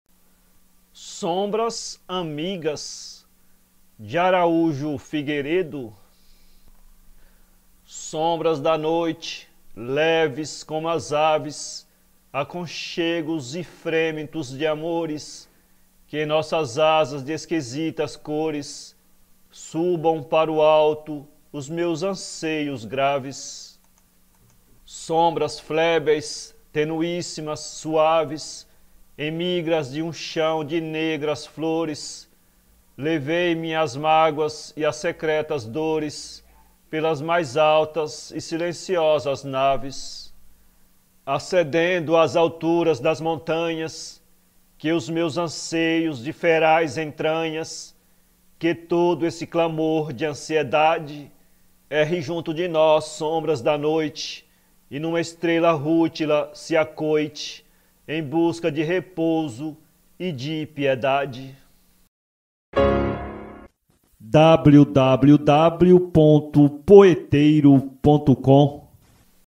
Sombras amigas (Poema, 1966), de Araújo Figueredo
Juvêncio de Araujo Figueredo nasceu no Desterro, Florianópolis, a 27 de setembro de 1864. Não chegou a completar nem as letras primárias. Não pôde desenvolver a inclinação natural para o desenho, porque, adolescente, perde a mãe e o pai, tornando-se ele o arrimo da família. Por longos anos, sua vida foi um peregrinar por diversos locais - Coqueiros, Rio de Janeiro, Tubarão, Tijucas, Santos, São Paulo, Campinas, Laguna - em diversificadas tentativas de trabalho: agricultura, olaria, pesca, jornalismo, promotor publico, tipógrafo, até, aos poucos, estabilizar-se no Congresso Estadual, onde posteriormente ascendeu ao cargo de subdiretor, exercendo as funções de diretor quando a morte o colheu, a 6 de abril de 1927. Seu poema, na inicial fase romântica, vem impregnado de forte sentimento da natureza. A seguir, irmanando-se ao Simbolismo de Cruz e Sousa, assume a temática da dor, do destino trágico e da morte, aspirando à transcendência. Finalmente, adota o Parnasianismo, burilando sonetos sobre a temática do mar.